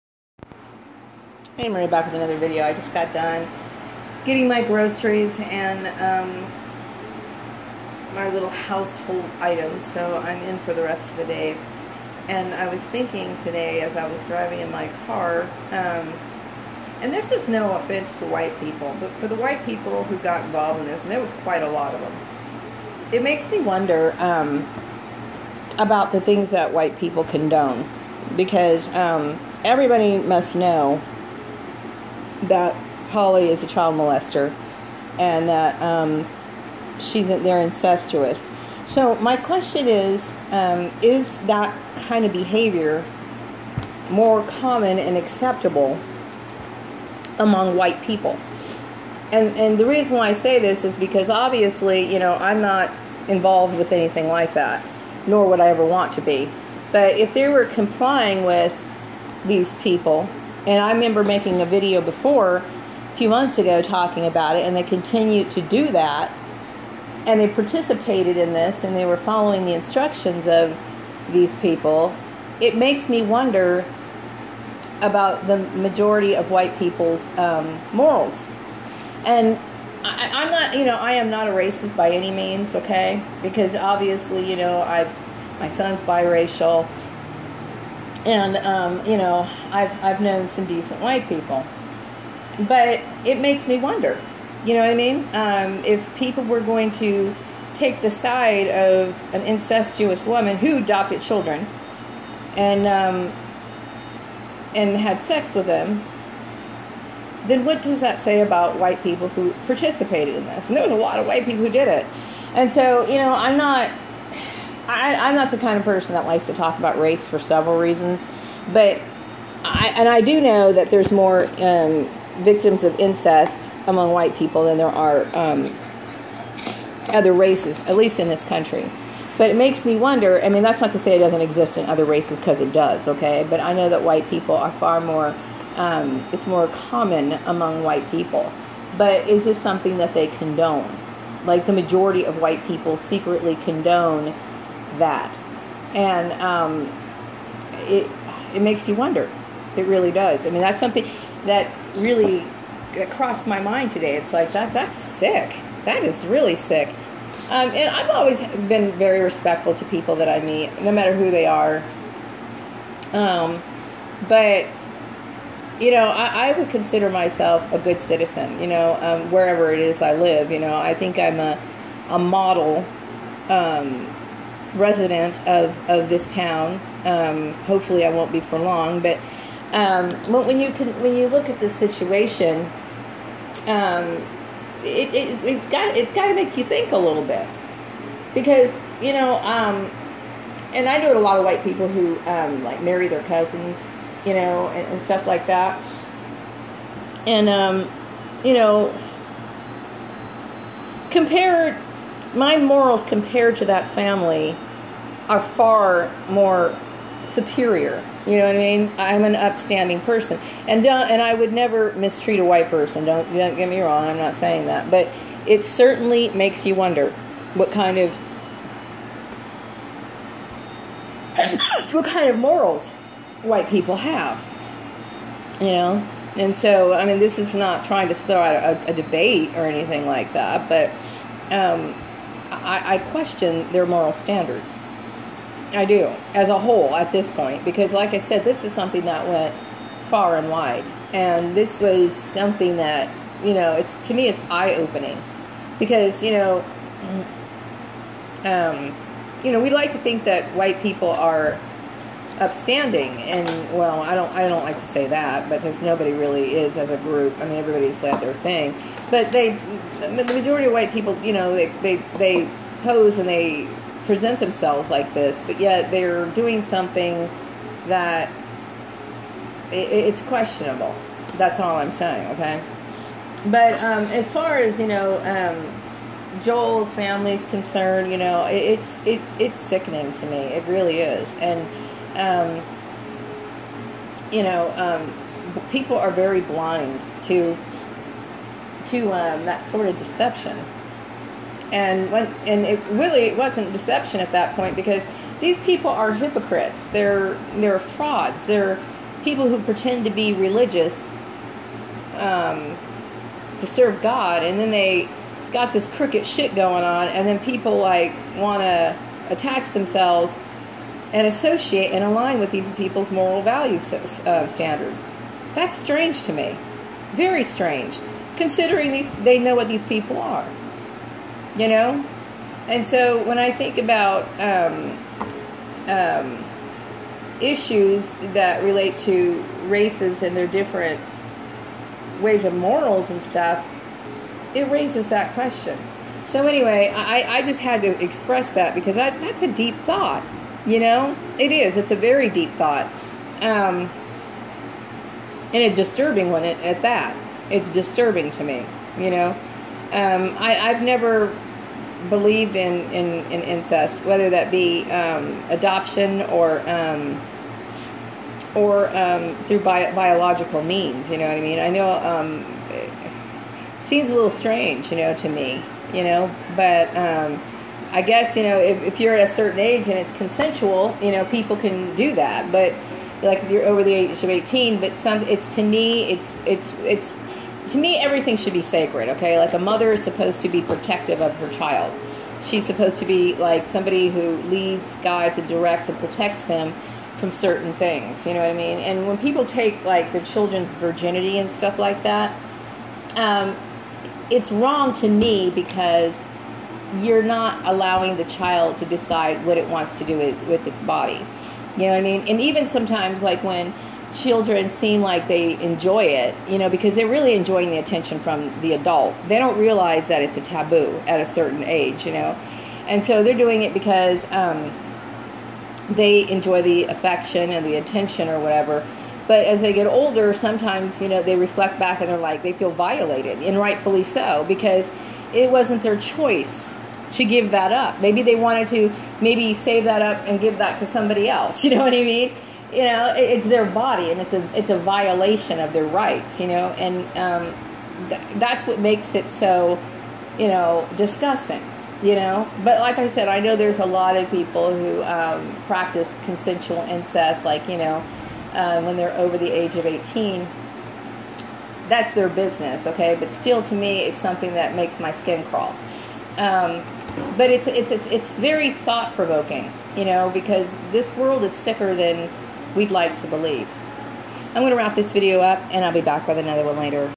Do White People Condone Incest & Child Molestation?
To consider the amount of people who participated in my harassment,  I can't help but to ask that question.